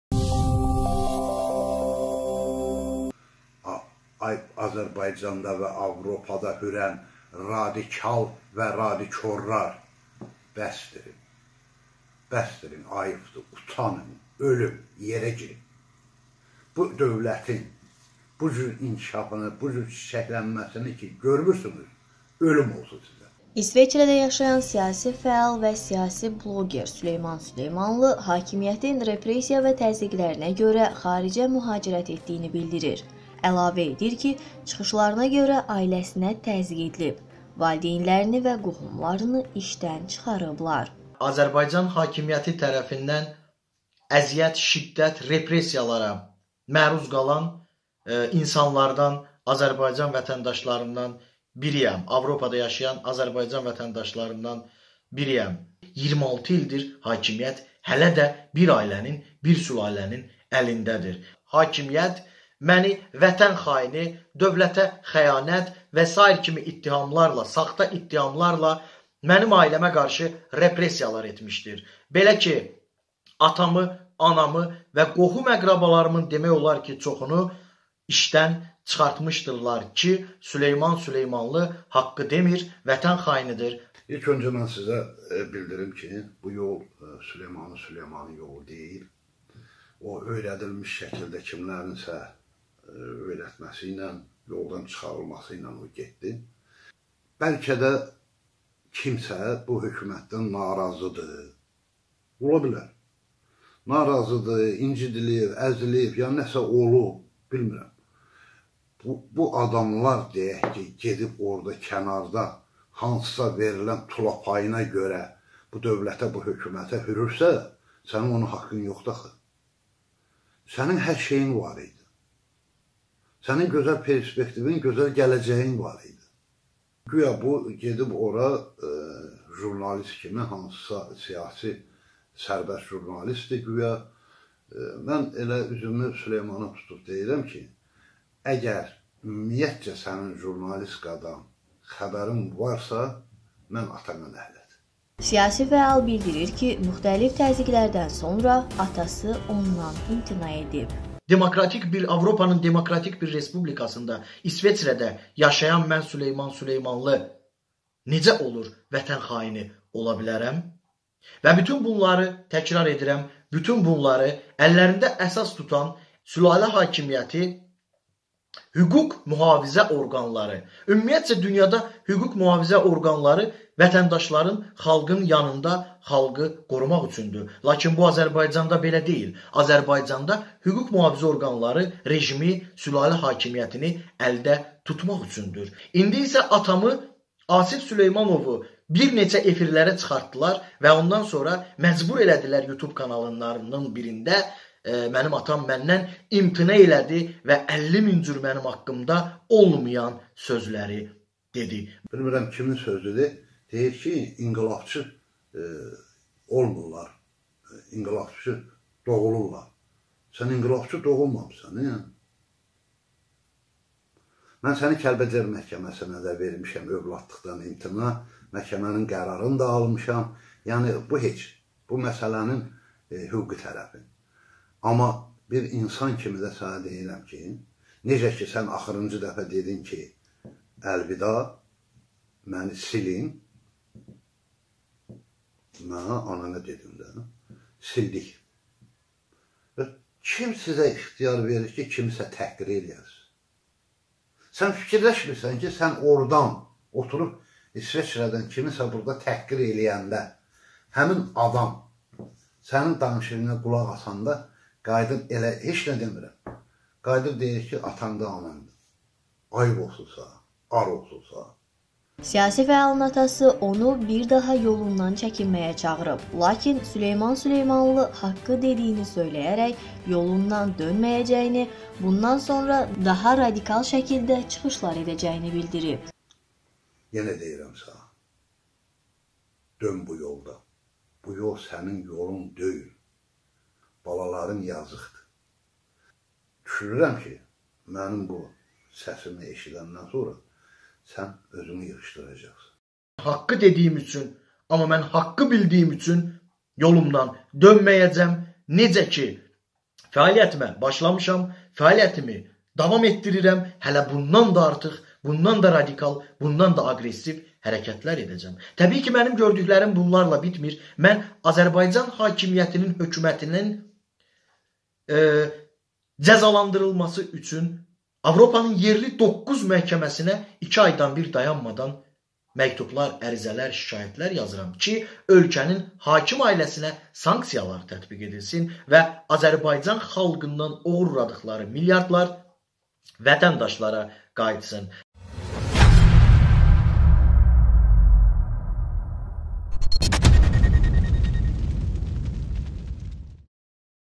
"Atam məndən imtina etsə də, mən dediyim doğrulardan imtina etməyəcəm"-blogger Süleyman Süleymanlı
"Sənin parlaq gələcəyin vardı, niyə narazı oldun sən bu hökümətdən?! Səni Kəlbəcər məhkəməsinə vermişik, səni ananla bərabər sildik oğulluqdan"-bloggerin atasının bu sözlərinin Süleyman Süleymanlı sifarişli olduğunu iddia edir

© KANAL13 [ Azərbaycanın ilk peşəkar internet televiziyası ] The First Internet TV of Azerbaijan mitinq 2019